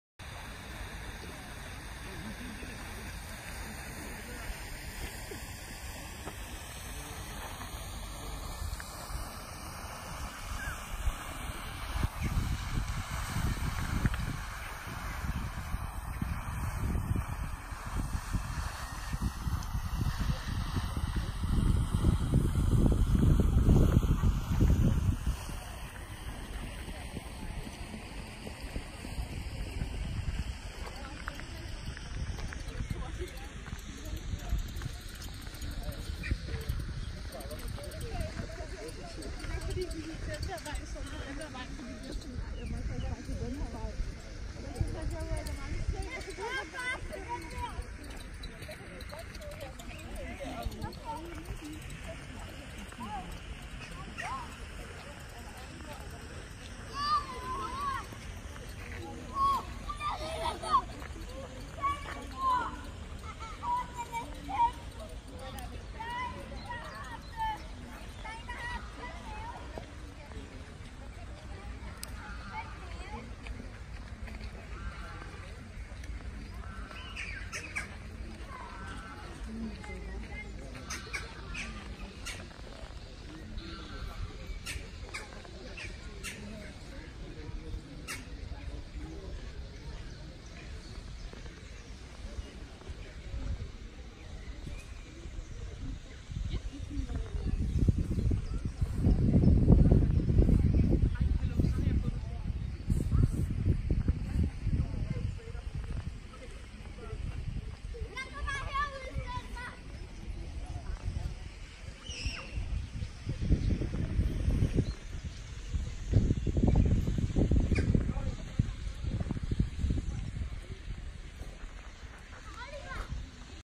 Denmark _Natural Beauty of Denmark
Frederiksberg Gardens  is one of the largest and most attractive greenspaces in Copenhagen, Denmark. Together with the adjacent Søndermarken it forms a green area of 64 hectares at the western edge of Inner Copenhagen. It is a romantic landscape garden designed in the English style.